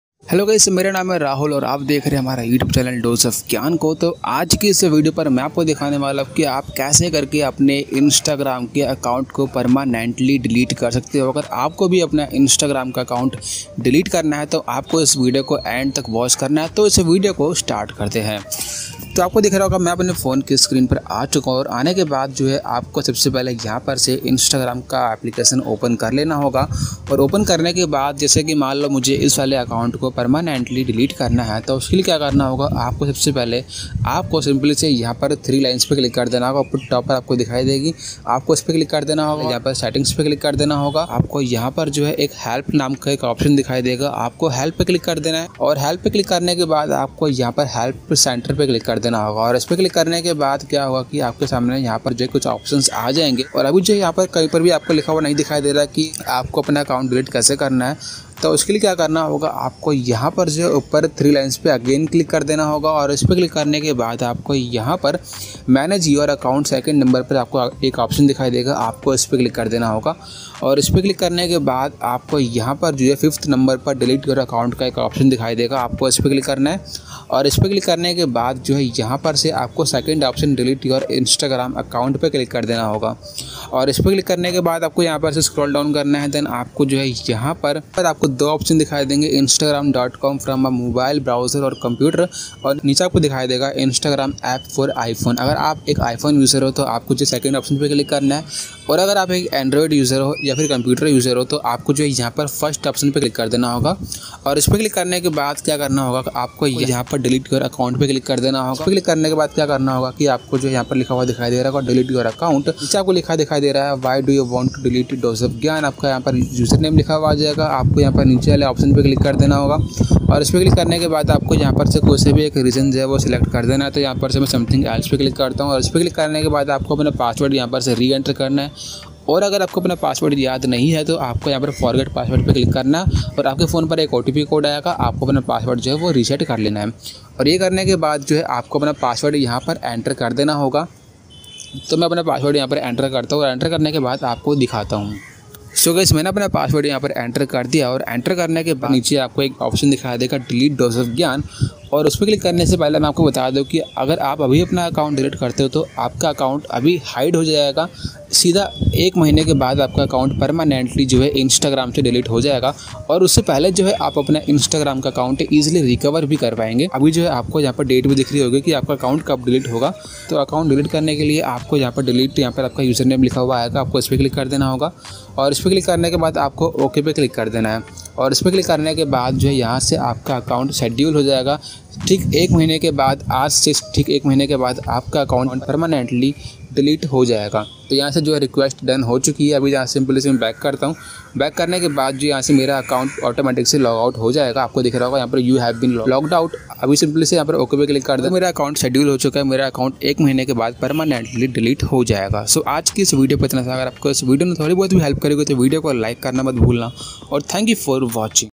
How to Permanently Delete instagram account,How to delete instagram account permanently 2022,Update
Your Queries...

How to permanently delete Instagram account

hello guys aaj ki is video par maine aapko btaya hai ki aap instagram account permanently delete kaise kar sakte hai and aapko pta hoga is process me kuch badlav aaya hai.

My Equipments⚡

• Mic {Original}
• Best Tripod For Beginners

Guys  you can follow me on ;-)

FACEBOOK. :-D